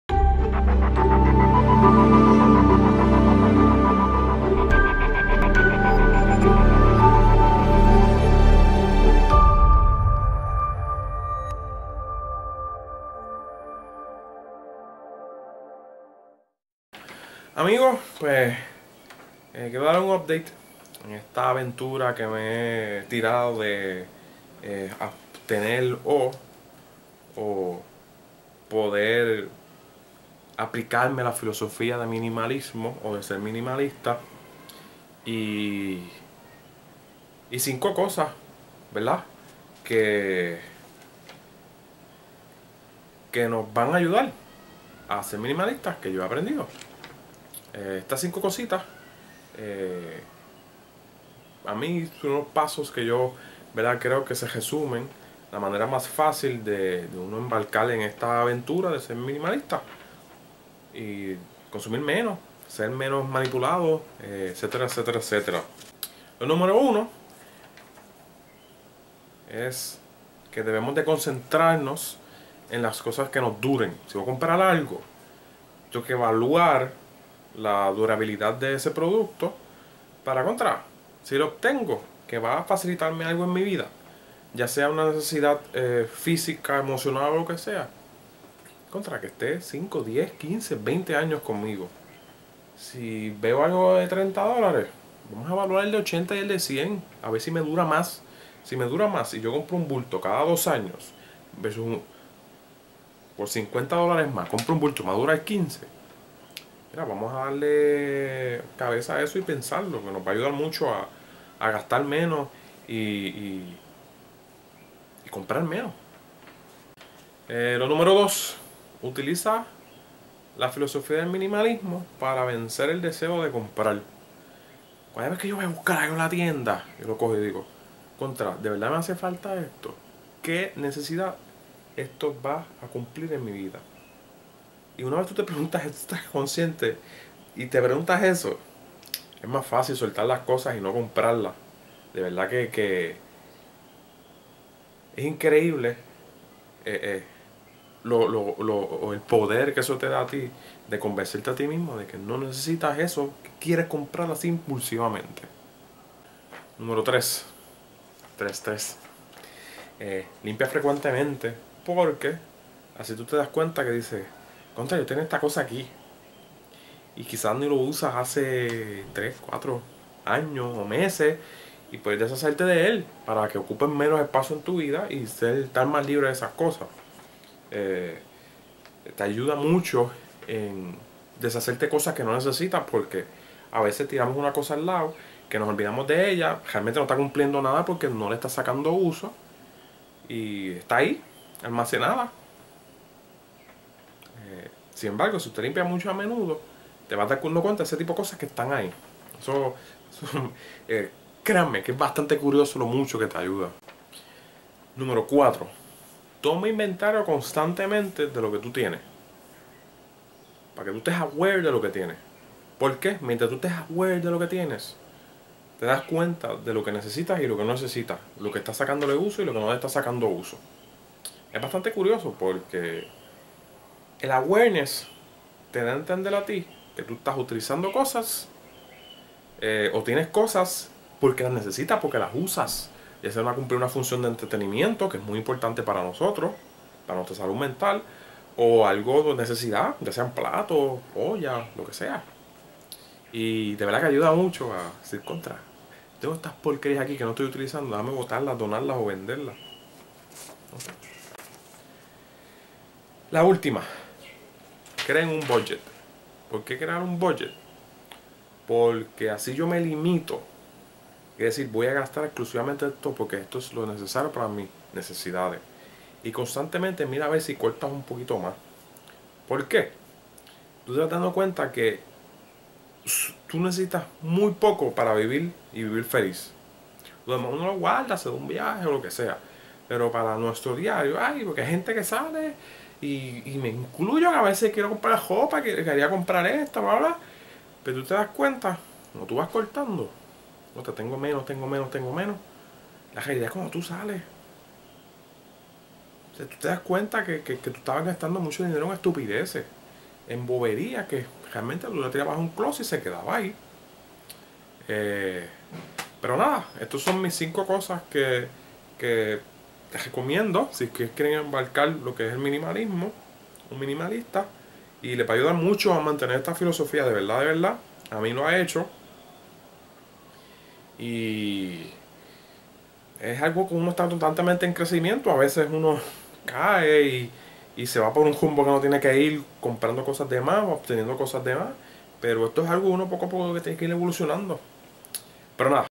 5 Cosas Que te Ayudaran a ser Minimalista
Dejo con ustedes 5 cosas que te ayudaran a perseguir la filosofia del minimalismo